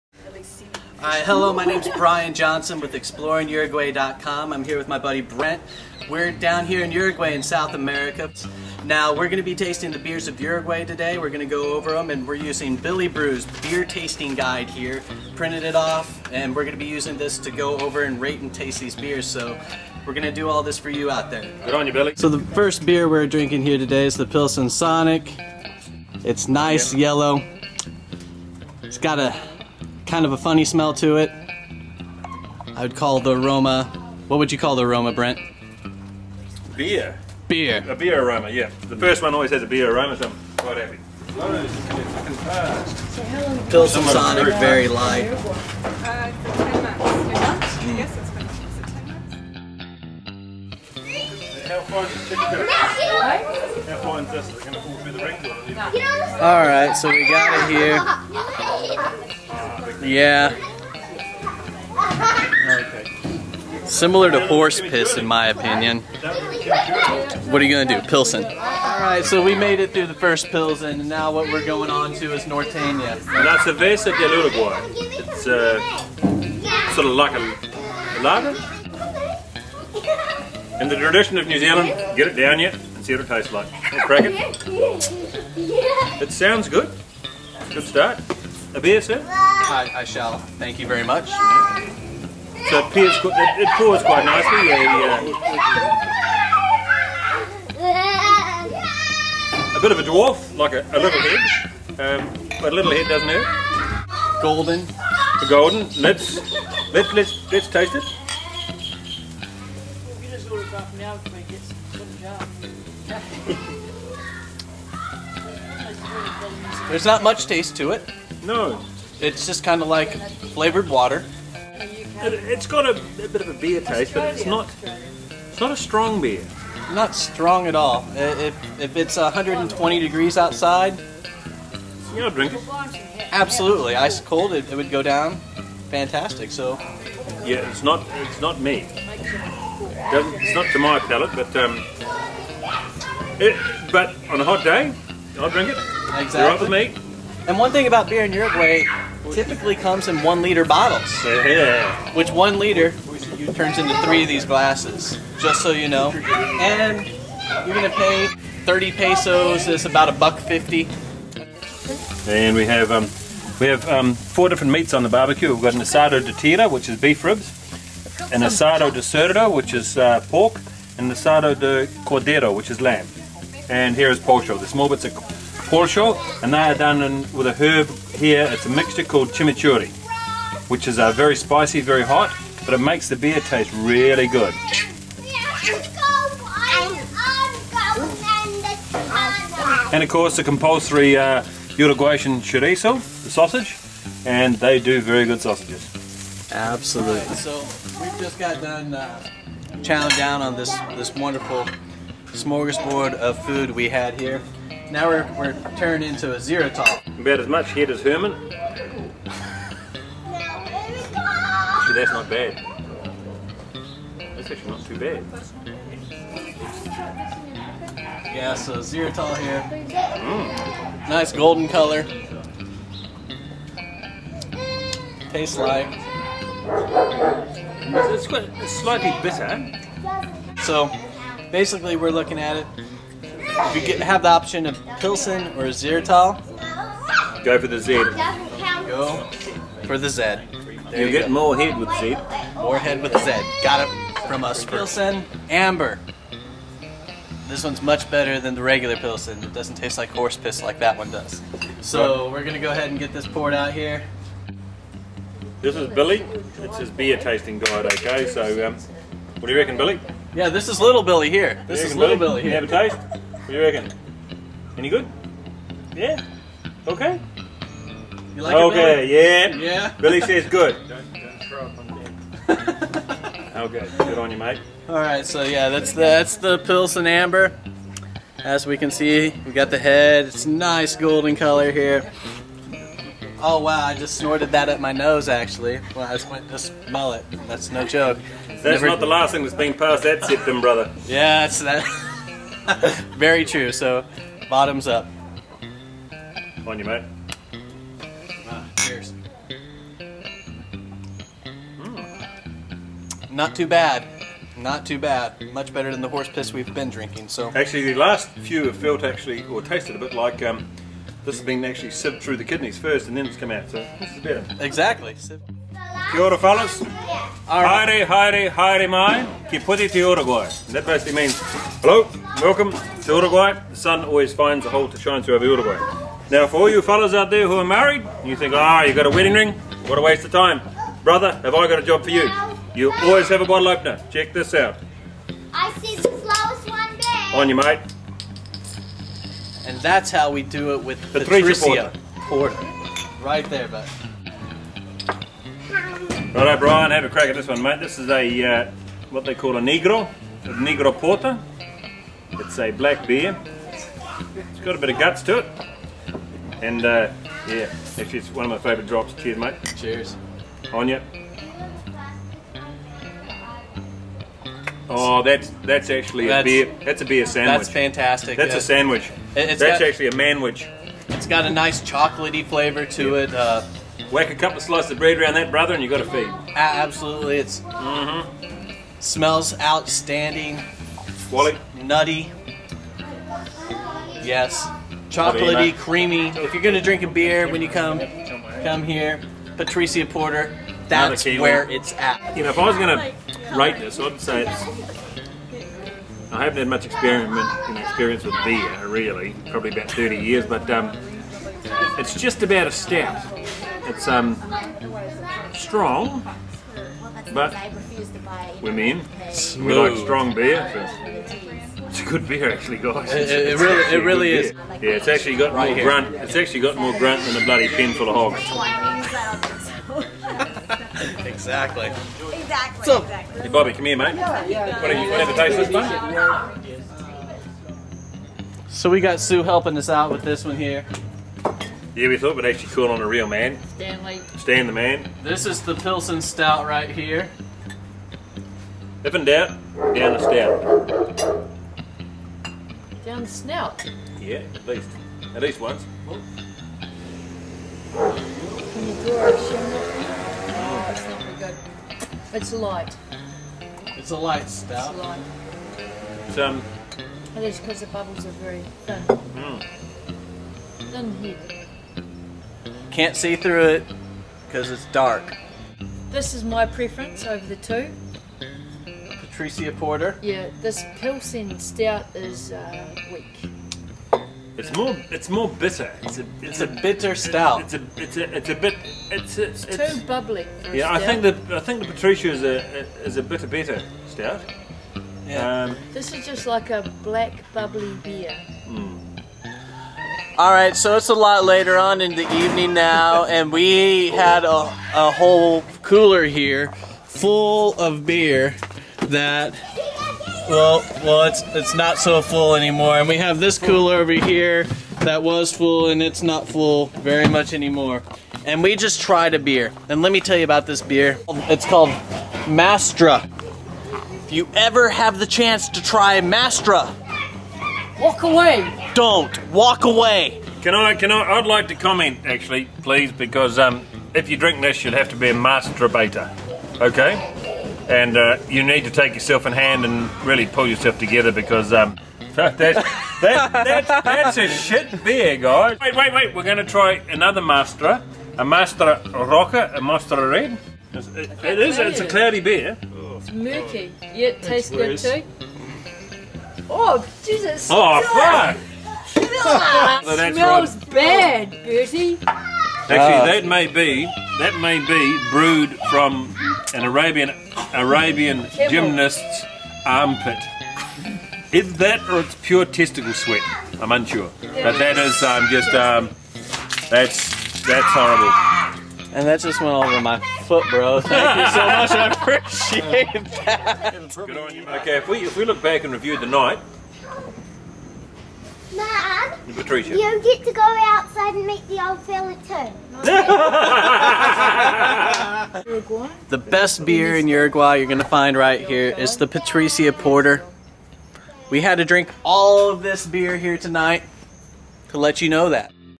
Exploring Uruguay's Beer.m4v
and Brent from New Zealand come together to do a taste testing of the beer in Uruguay

From sober to drunk in 10 minutes... and having a blast doing it...